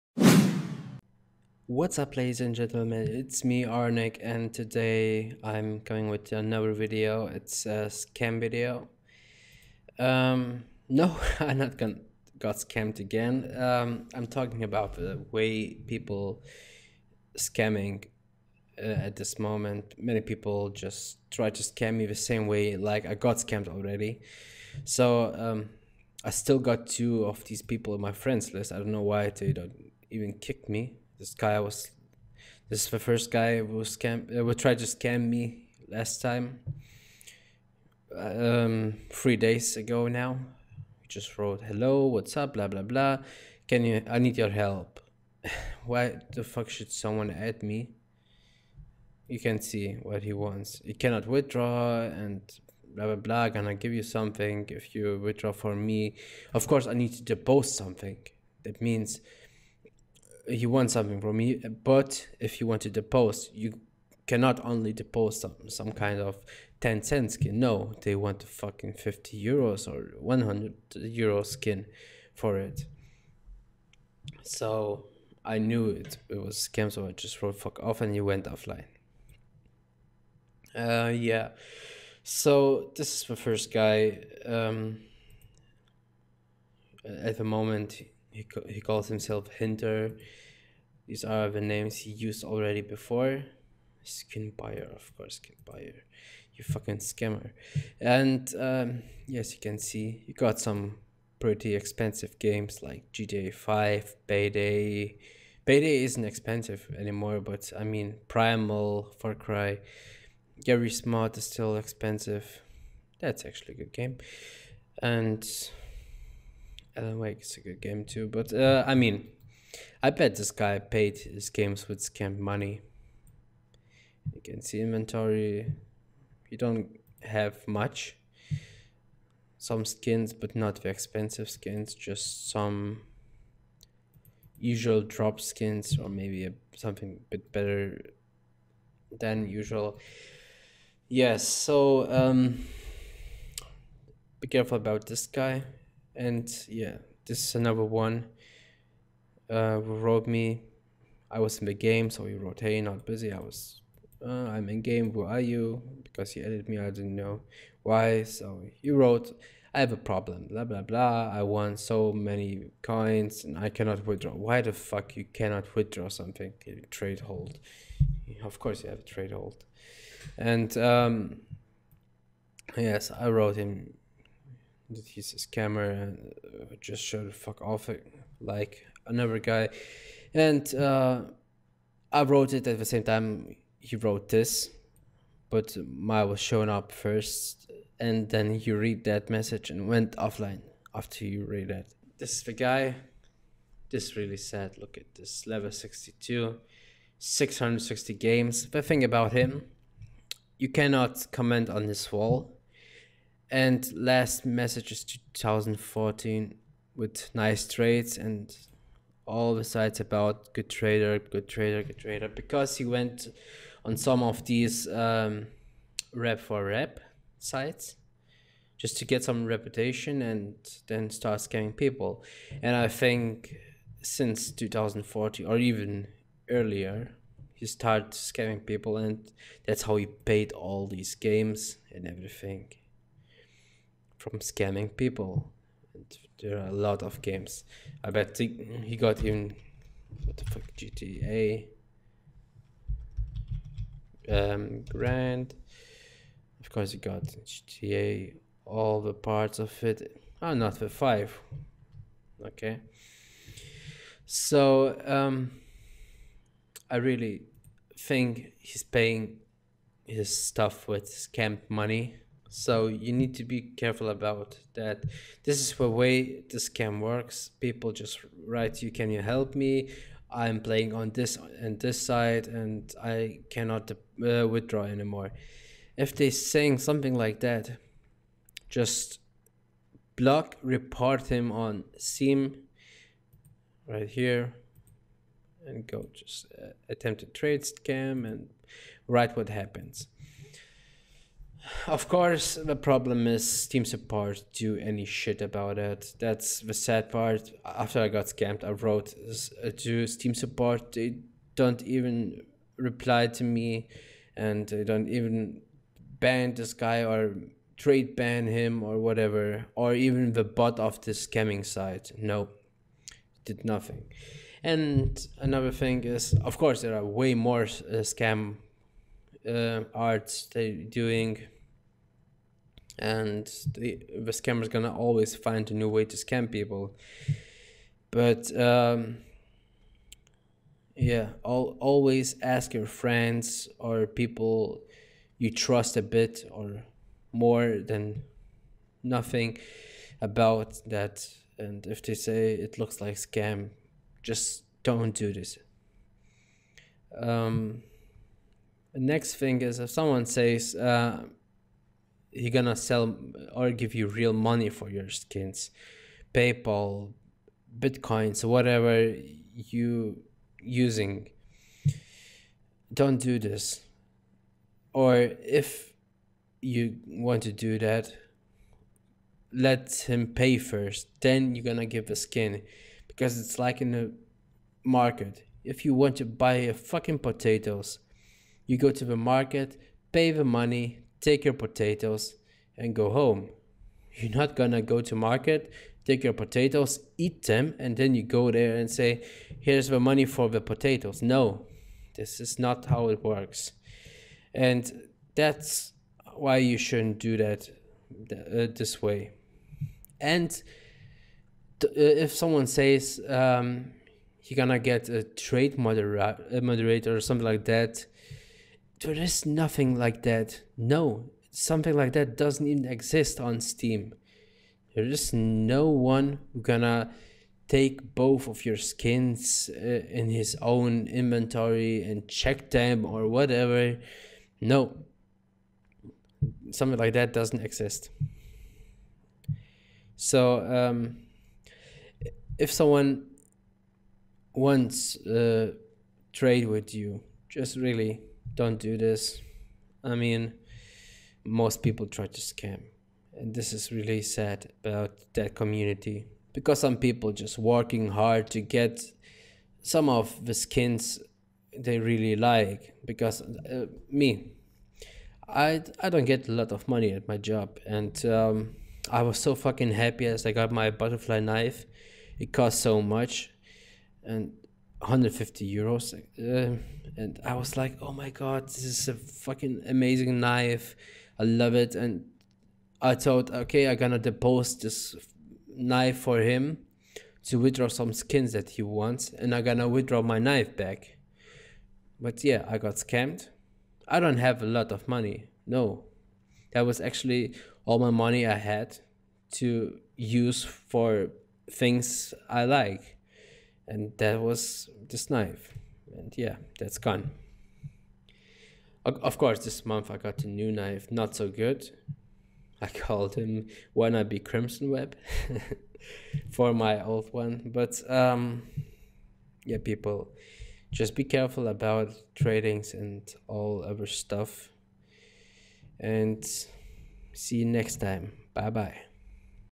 Be careful with that scam !!!
In this video i want to show you, what way some of scammers are using to scam other people.
Allready 6 people tryed to scam me same way.
Sadly steam support do nothing about that, so maybe some people gonna be careful after watching this